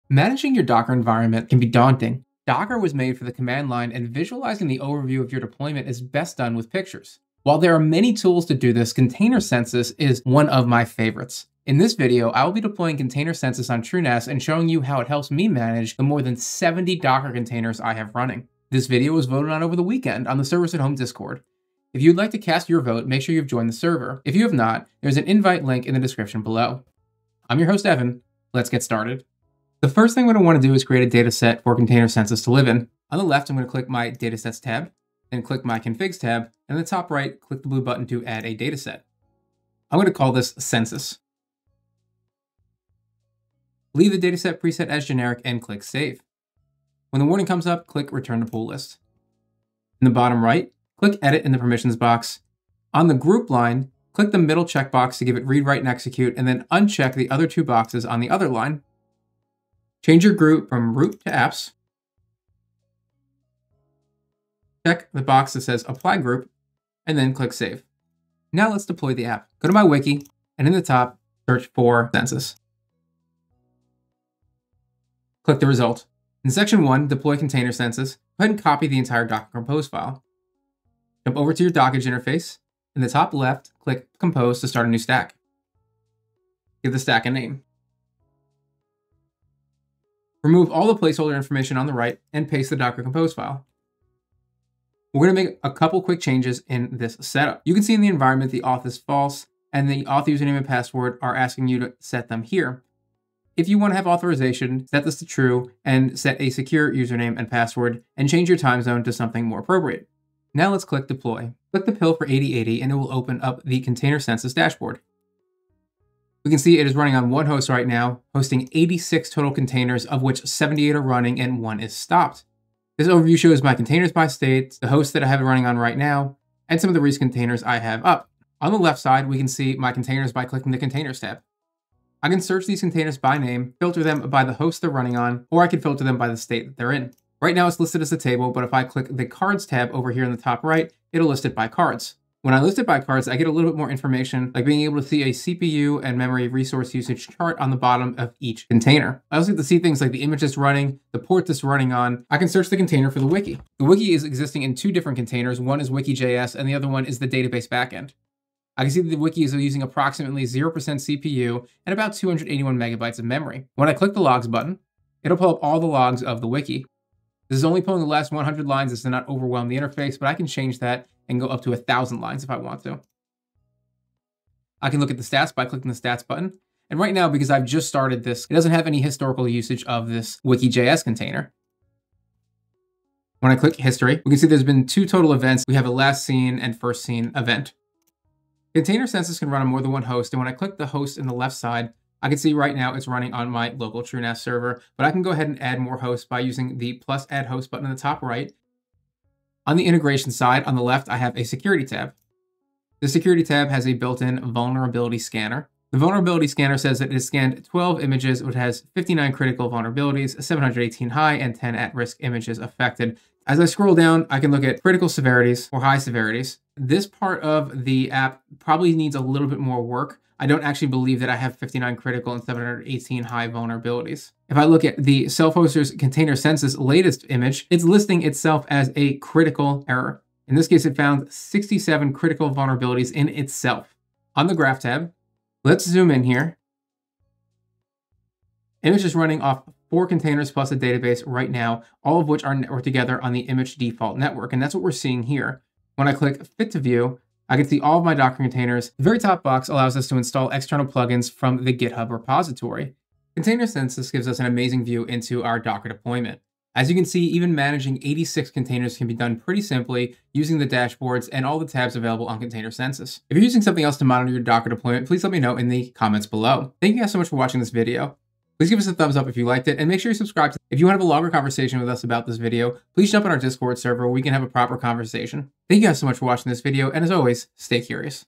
Installing Container Census | TrueNAS Community Edition 2025
In this video, we take a look at Container Census, a lightweight Go-based tool that scans your Docker environment and gives you a clear, historical view of every container running across one or multiple hosts. Instead of guessing what’s deployed or manually tracking changes, Container Census keeps an ongoing record of your container landscape.

I’ll walk through how it works, what data it collects, and why having historical container visibility matters — especially in growing homelabs and multi-host Docker setups. If you want better insight into what’s running, where it’s running, and how things change over time, this tool is worth checking out.

📌 Timestamps:
0:00 Intro and overview
0:28 Install and setup on TrueNAS
1:51 Dashboard walkthrough and demo
4:14 Final thoughts and wrap-up